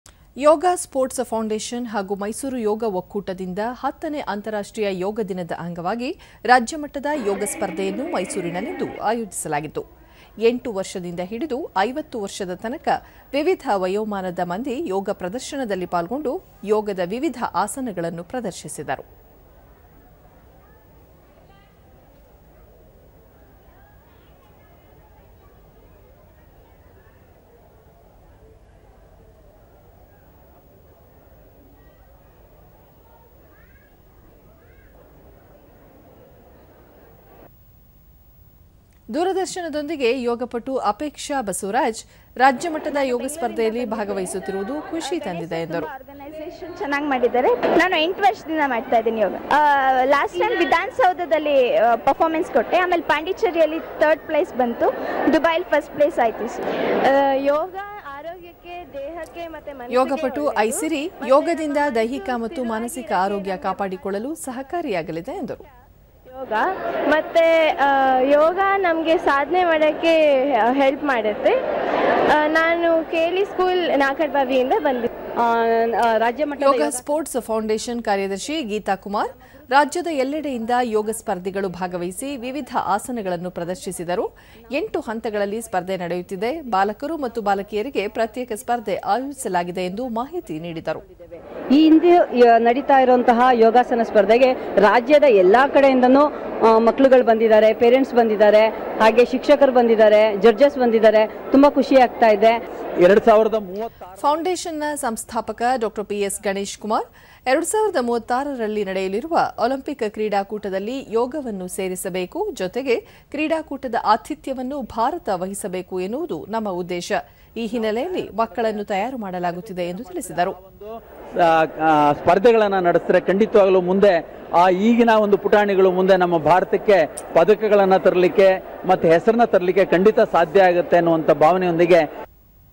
10ನೇ ಅಂತಾರಾಷ್ಟ್ರೀಯ ಯೋಗ ದಿನಾಚರಣೆ ; ಮೈಸೂರಿನಲ್ಲಿ ರಾಜ್ಯಮಟ್ಟದ ಯೋಗ ಸ್ಪರ್ಧೆ ಆಯೋಜನೆ
ಯೋಗ ಸ್ಫೋರ್ಟ್ಸ್ ಫೌಂಡೇಷನ್ ಹಾಗೂ ಮೈಸೂರು ಯೋಗ ಒಕ್ಕೂಟದಿಂದ 10ನೇ ಅಂತಾರಾಷ್ಟ್ರೀಯ ಯೋಗ ದಿನದ ಅಂಗವಾಗಿ ರಾಜ್ಯಮಟ್ಟದ ಯೋಗ ಸ್ಪರ್ಧೆಯನ್ನು ಮೈಸೂರಿನಲ್ಲಿಂದು ಆಯೋಜಿಸಲಾಗಿತ್ತು.
8 ವರ್ಷದಿಂದ ಹಿಡಿದು 50 ವರ್ಷದ ತನಕ ವಿವಿಧ ವಯೋಮಾನದ ಮಂದಿ ಯೋಗ ಪ್ರದರ್ಶನದಲ್ಲಿ ಪಾಲ್ಗೊಂಡು ಯೋಗದ ವಿವಿಧ ಆಸನಗಳನ್ನು ಪ್ರದರ್ಶಿಸಿದರು.
(WATCH)
ದೂರದರ್ಶನದೊಂದಿಗೆ ಯೋಗಪಟು ಅಪೇಕ್ಷಾ ಬಸವರಾಜ್,ರಾಜ್ಯಮಟ್ಟದ ಯೋಗ ಸ್ಪರ್ಧೆಯಲ್ಲಿ ಭಾಗವಹಿಸುತ್ತಿರುವುದು ಖುಷಿ ತಂದಿದೆ ಎಂದರು.
(HOLD)
ಮತ್ತೊಬ್ಬ ಯೋಗಪಟು ಐಸಿರಿ, ಯೋಗದಿಂದ ದೈಹಿಕ ಮತ್ತು ಮಾನಸಿಕ ಆರೋಗ್ಯ ಕಾಪಾಡಿಕೊಳ್ಳಲು ಸಹಕಾರಿಯಾಗಲಿದೆ ಎಂದು ತಿಳಿಸಿದರು.
(HOLD)
ಯೋಗಾ ಸ್ಫೋರ್ಟ್ಸ್ ಫೌಂಡೇಷನ್ ಕಾರ್ಯದರ್ಶಿ ಗೀತಾ ಕುಮಾರ್, ರಾಜ್ಯದ ಎಲ್ಲೆಡೆಯಿಂದ ಯೋಗ ಸ್ಪರ್ಧಿಗಳು ಭಾಗವಹಿಸಿ ವಿವಿಧ ಆಸನಗಳನ್ನು ಪ್ರದರ್ಶಿಸಿದರು. ಎಂಟು ಹಂತಗಳಲ್ಲಿ ಸ್ಪರ್ಧೆ ನಡೆಯುತ್ತಿದೆ. ಬಾಲಕರು ಮತ್ತು ಬಾಲಕಿಯರಿಗೆ ಪ್ರತ್ಯೇಕ ಸ್ಪರ್ಧೆ ಆಯೋಜಿಸಲಾಗಿದೆ ಎಂದು ಹೇಳಿದರು.
(HOLD)
ಫೌಂಡೇಷನ್ ನ ಸಂಸ್ಥಾಪಕ ಡಾ. ಪಿ.ಎಸ್. ಗಣೇಶ್ ಕು ಮಾರ್, 2036ರಲ್ಲಿ ನಡೆಯಲಿರುವ ಒಲಿಂಪಿಕ್ ಕ್ರೀಡಾಕೂಟದಲ್ಲಿ ಯೋಗವನ್ನು ಸೇರಿಸಬೇಕು. ಜೊತೆಗೆ ಕ್ರೀಡಾಕೂಟದ ಆತಿಥ್ಯವನ್ನು ಭಾರತ ವಹಿಸಬೇಕು ಎನ್ನುವುದು ನಮ್ಮ ಉದ್ದೇಶ. ಈ ಹಿನ್ನೆಲೆಯಲ್ಲಿ ಮಕ್ಕಳನ್ನು ತಯಾರು ಮಾಡಲಾಗುತ್ತಿದೆ ಎಂದು ತಿಳಿಸಿದರು.
(HOLD)